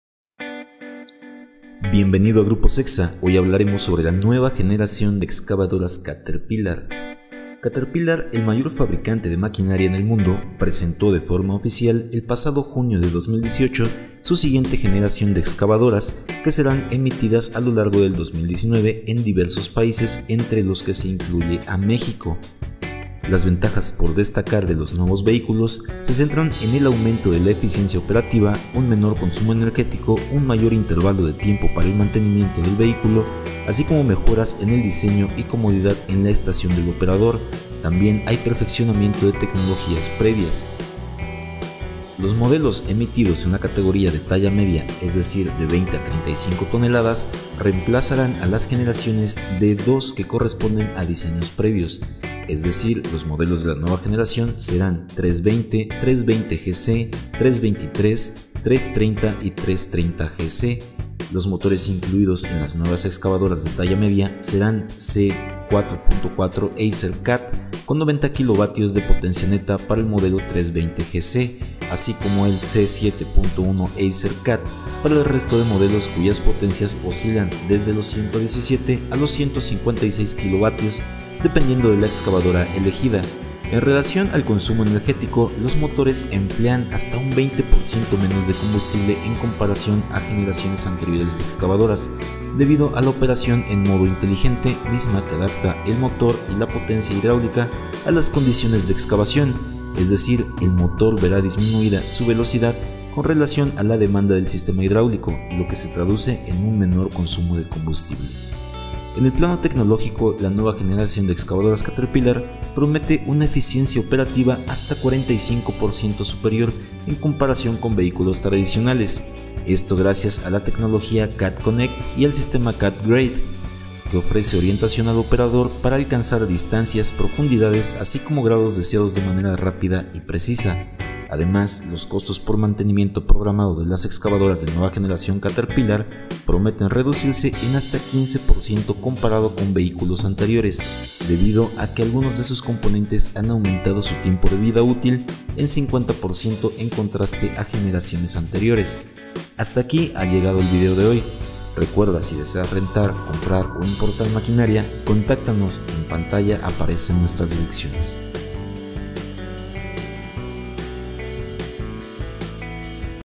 REVIEW Nueva generación excavadoras caterpillar
La nueva generación de excavadoras de talla media Caterpillar es una de las mejores opciones en el mercado en relación a consumo energético y eficiencia.
Derechos:
Caterpillar, [Cat® Products]. (2019, Febrero 11). 320 GC, 320, 323 - Next Generation Cat® Excavators: More Choices to Match Your Work [Archivo de video]. Recuperado de

Visita nuestros sitios:
Renta: goo.gl/ZKf1Nt
Compra: goo.gl/Ys2cGQ
Página oficial en Facebook
Únete a nuestro equipo:
goo.gl/FkwwYf